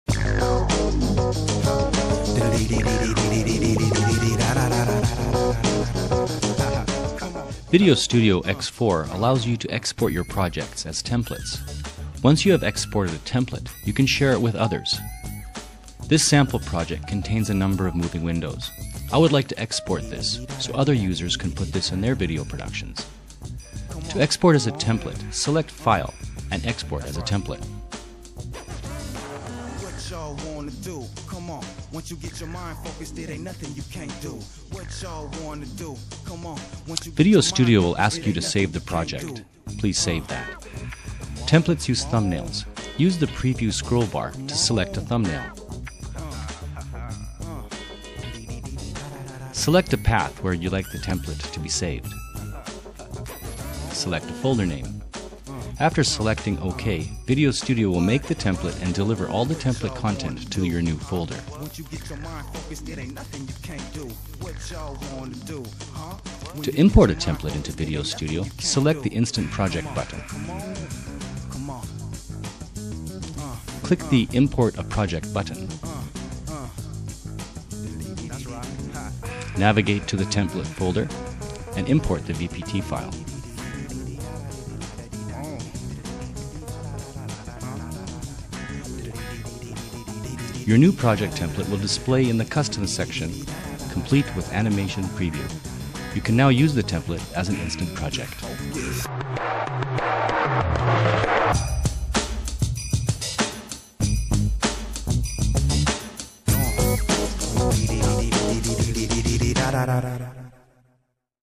Exporting Templates from Corel VideoStudio Pro X4
VideoStudio Pro X4 offers new Instant Movie capabilities, including the ability to create, import and export movie templates. Make a template of cool title sequences, effects or complete movies, then reuse or share with others.